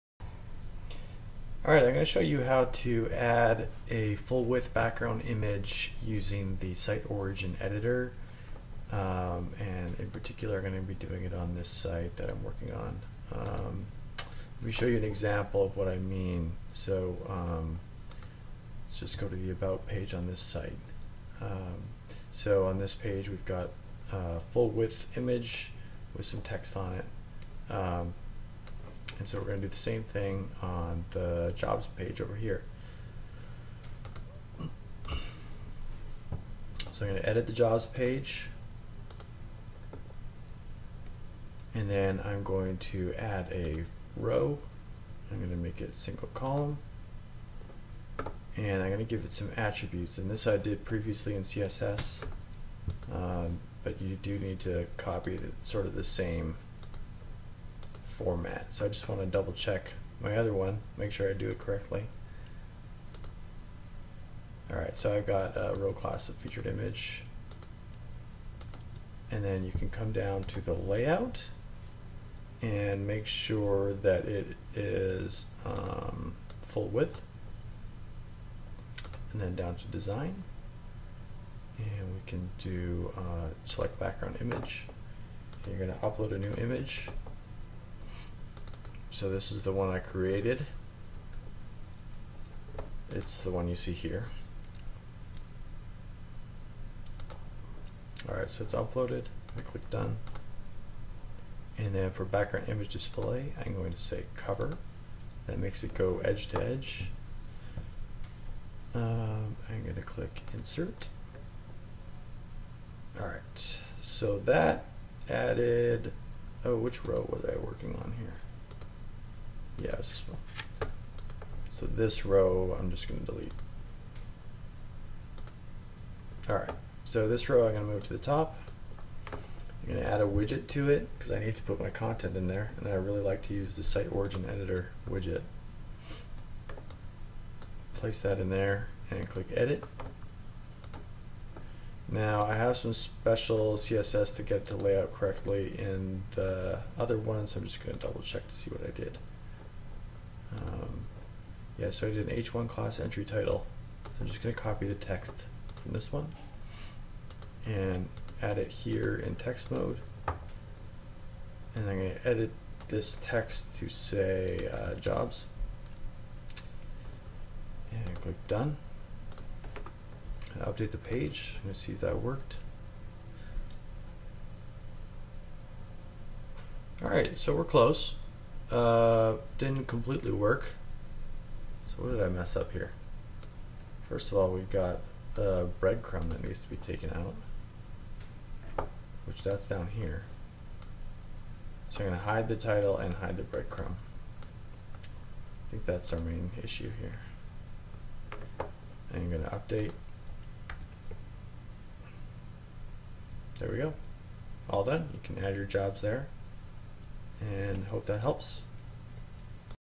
How to add a full width background image using Page Builder by SiteOrigin
A quick demo of how to add a full width background image to a wordpress website